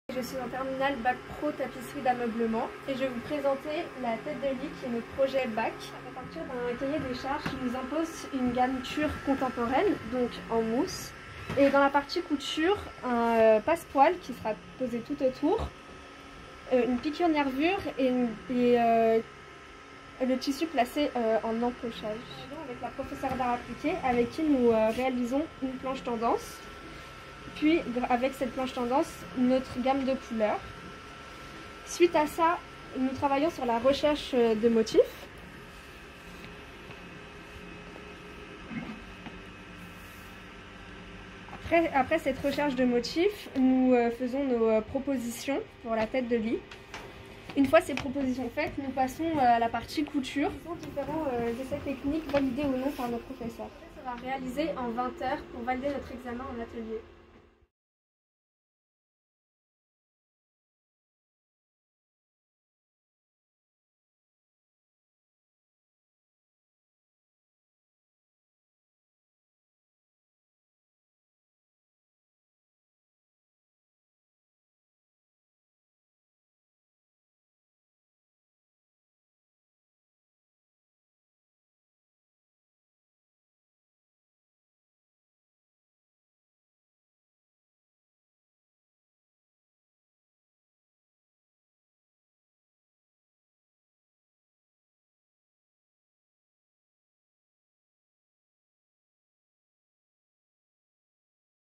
Bac Pro Tapisserie réalisation d'une tête de lit avec garniture contemporaine en mousse
Présentation par Esther, une élève de Bac Pro Tapisserie  du Lycée La Source du projet de  réalisation d'une tête de lit avec garniture contemporaine en mousse